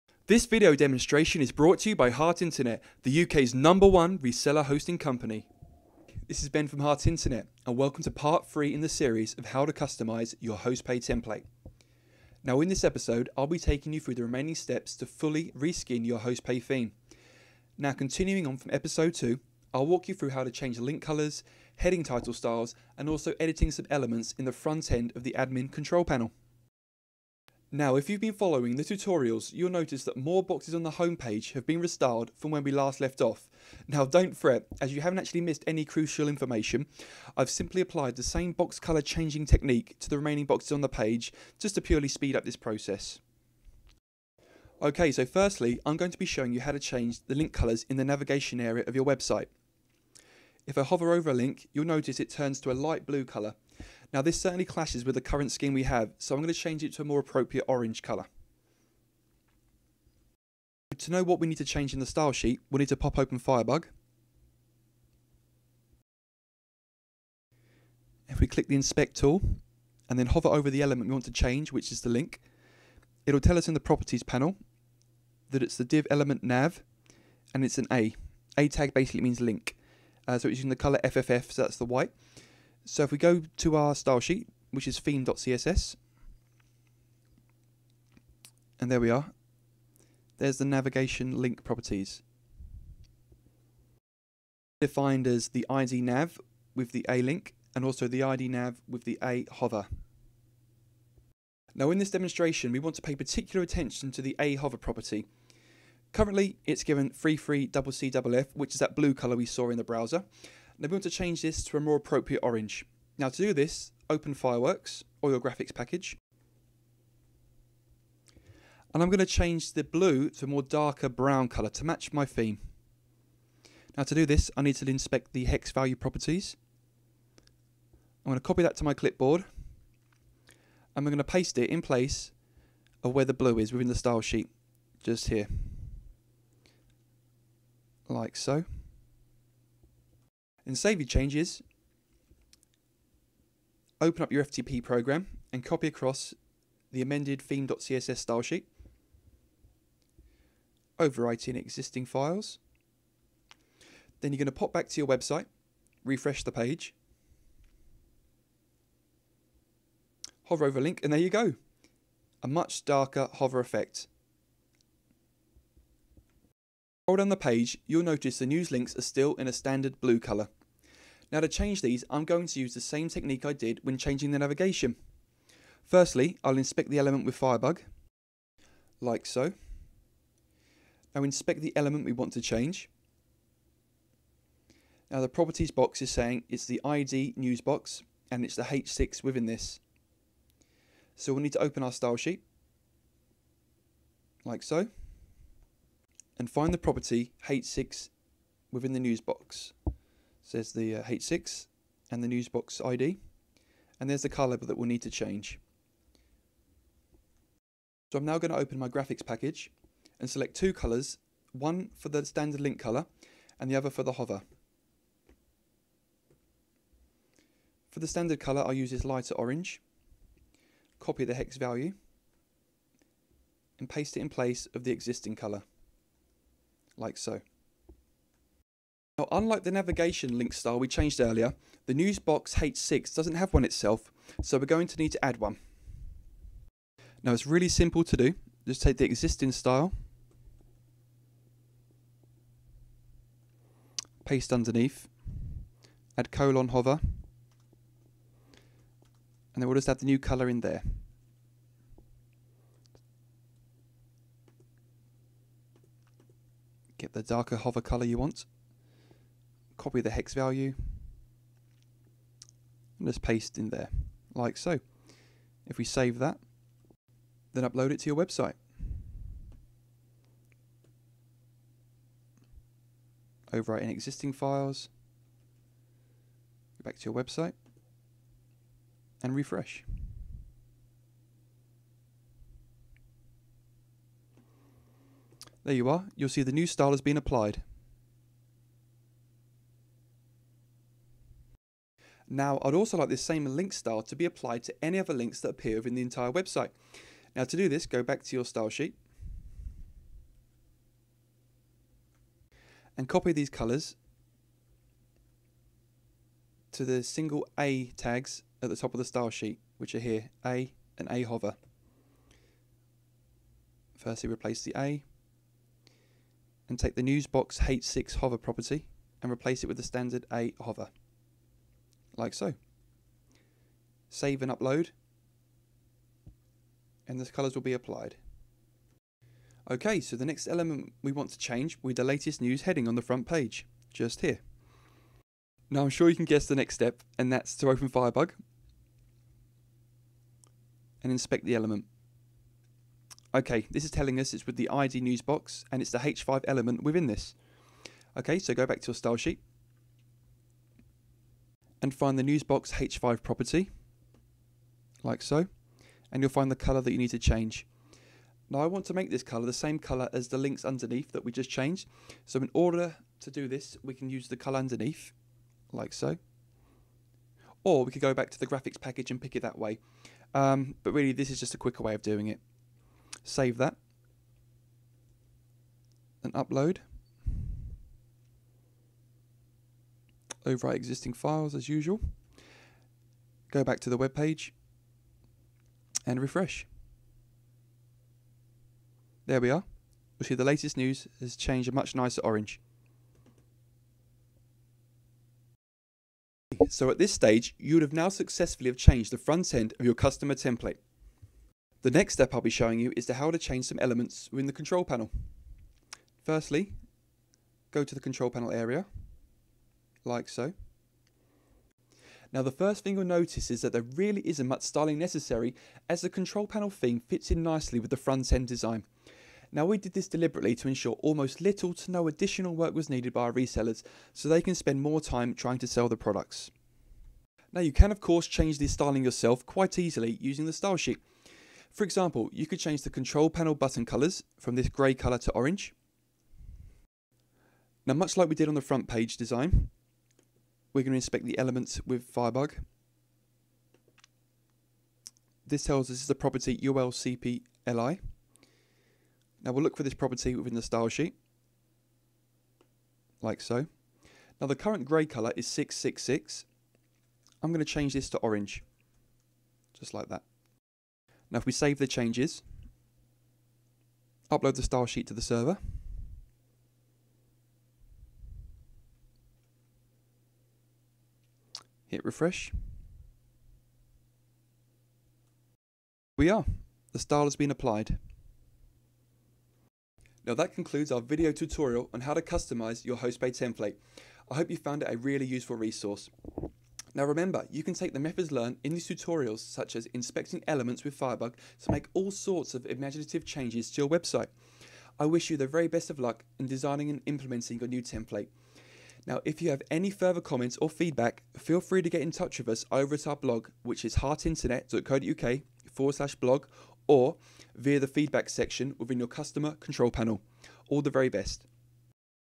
Customise HostPay Template: Part 3 - CSS Link & Heading Colours
Video tutorial on how to customise a HostPay template. How to change link and heading colours, also a brief overview on styling the contorl panel - HostPay Web Hosting Shopping Cart System